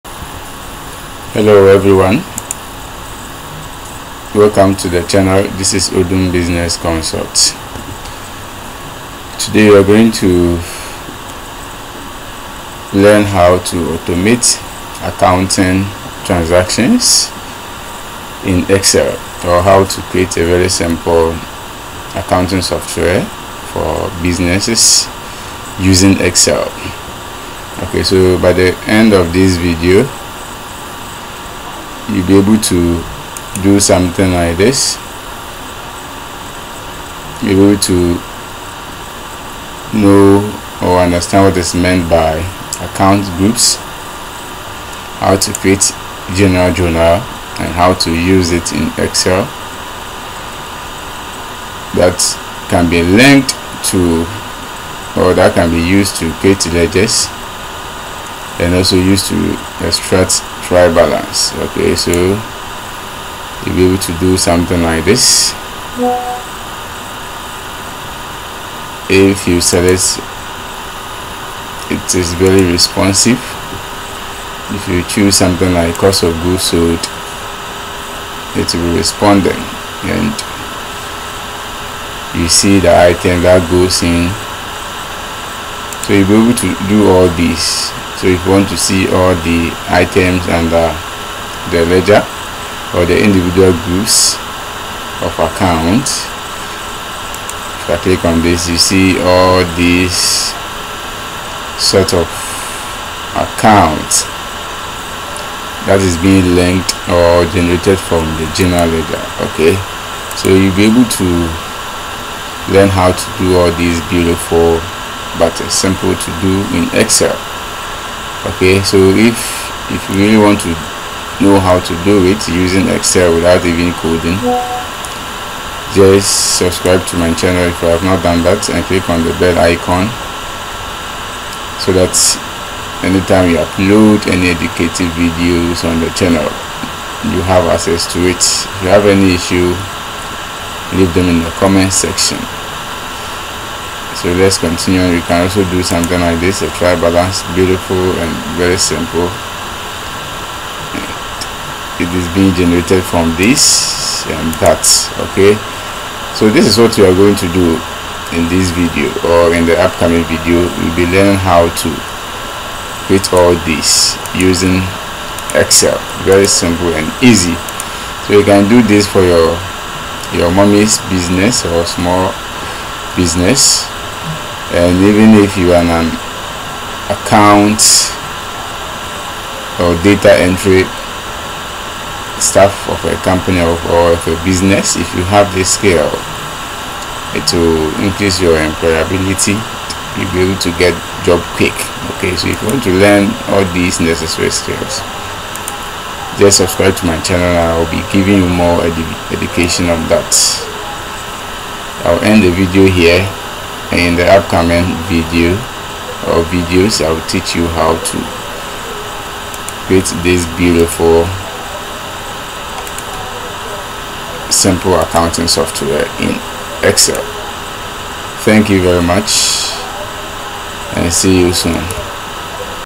How to Automate Accounting Ledger, Trial Balance in Excel
This video teaches how to create an Accounting Software for small Business using MS Excel

Financial accounting